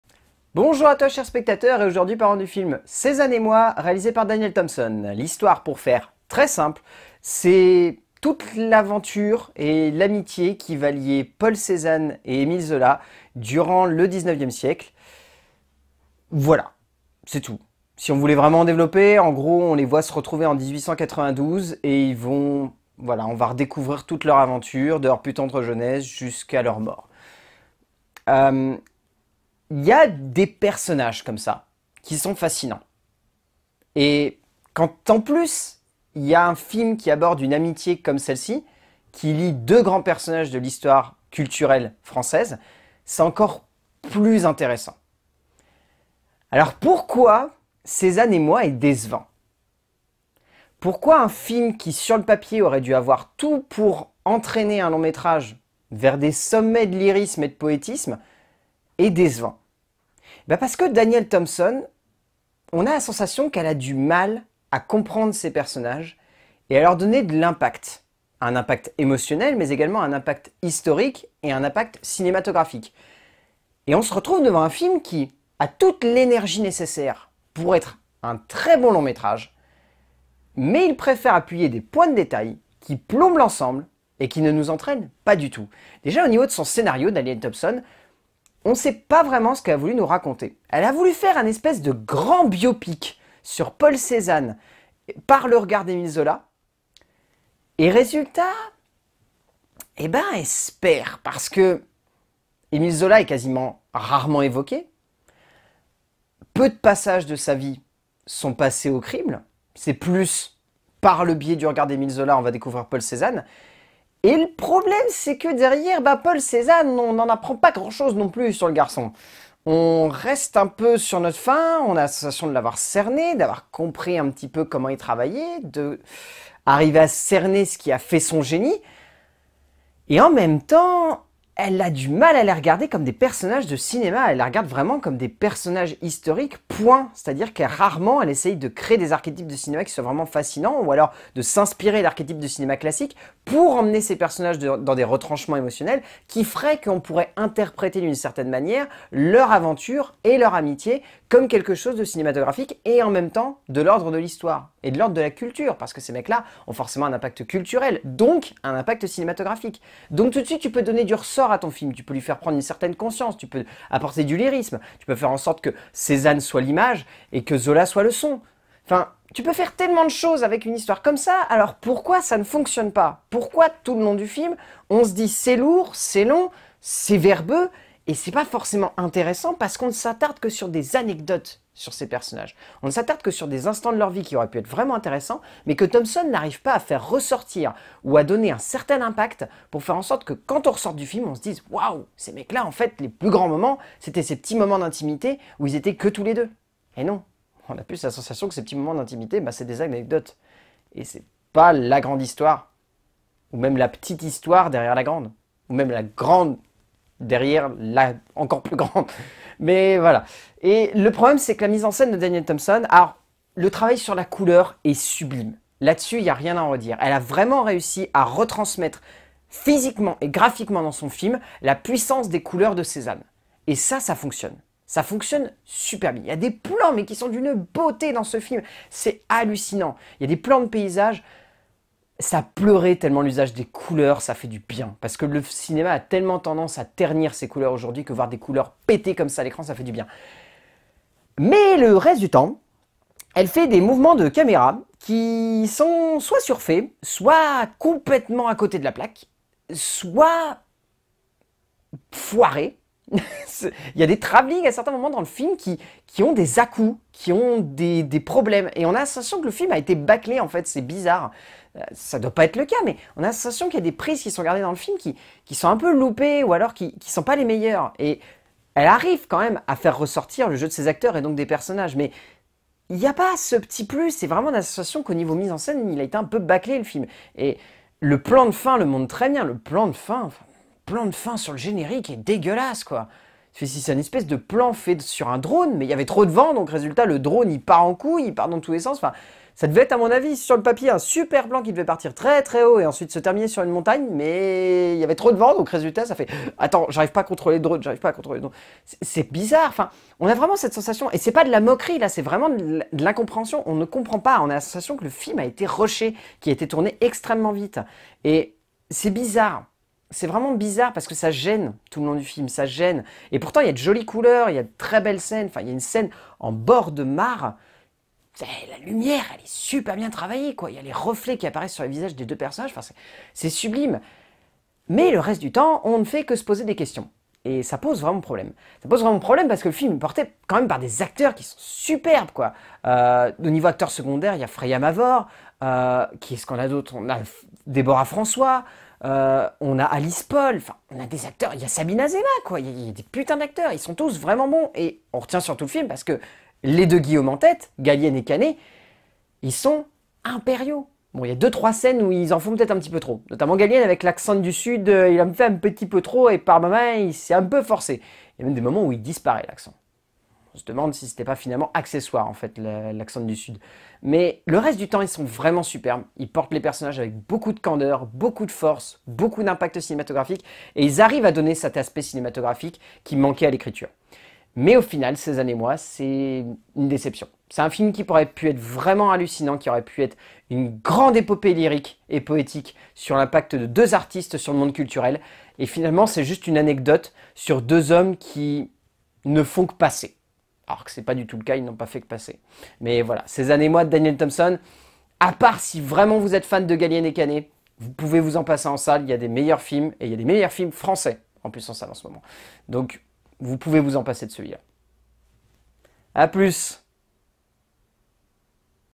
LCDC - Cézanne et Moi
Vlog critique sur la sortie d'un portrait hors du commun, sur deux auteurs de génie, ayant inspiré le monde de l'art, et ayant vécu une amitié unique et tumultueuse, "Cézanne et Moi"